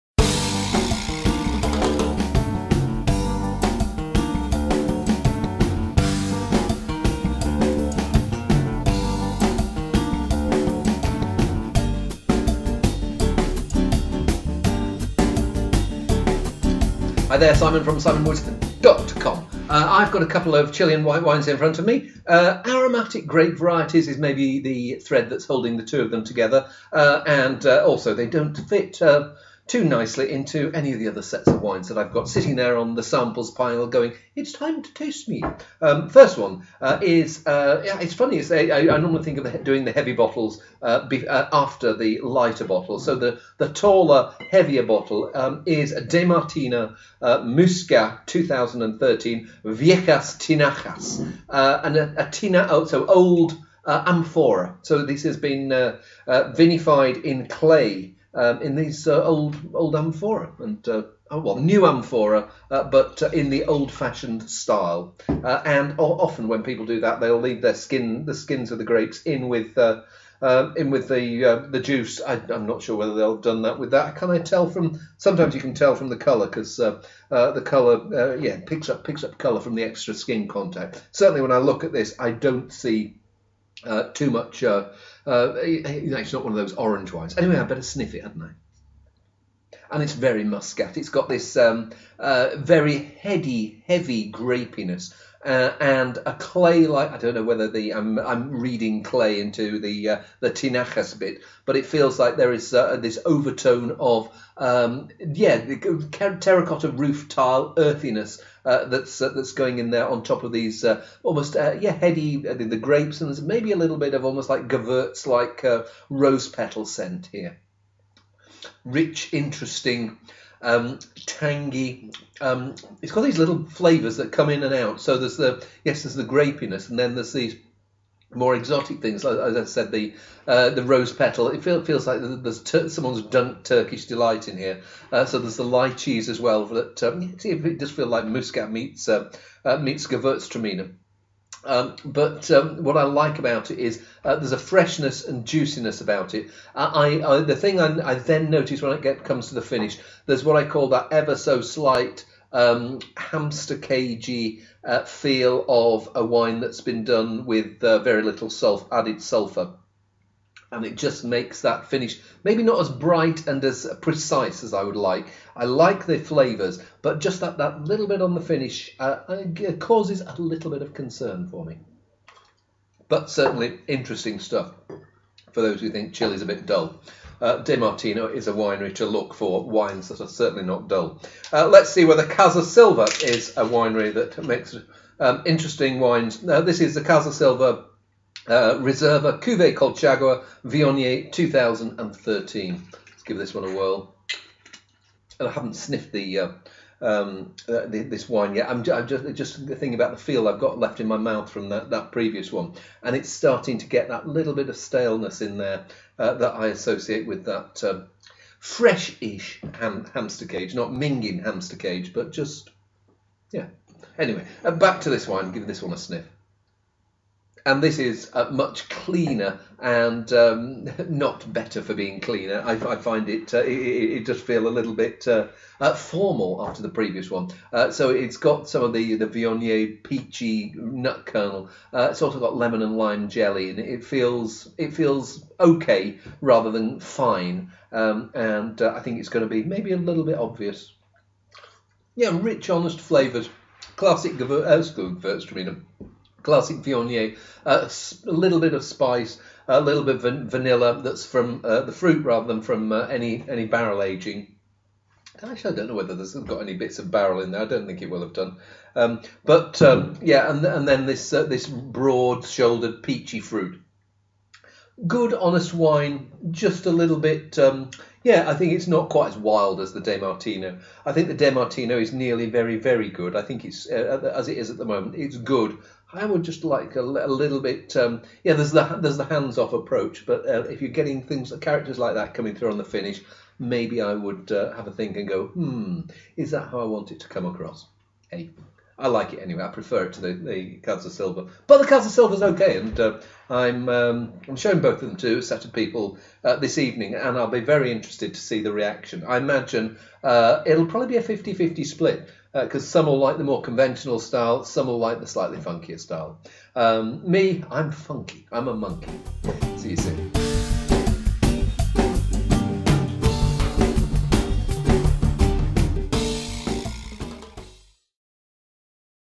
Wine Tasting with Simon Woods: Two Aromatic Chilean Whites
Simon Woods tries two Chilean wines, a Viognier and first, a Muscat made in clay amphorae...

The wines tasted in this video are:

De Martino Viejas Tinajas Muscat 2013, Itata Valley, Chile (£13-£14 Les Caves de Pyrene, Whole Foods Market, Winemakers Club, Exel Wines, Bijou Bottles)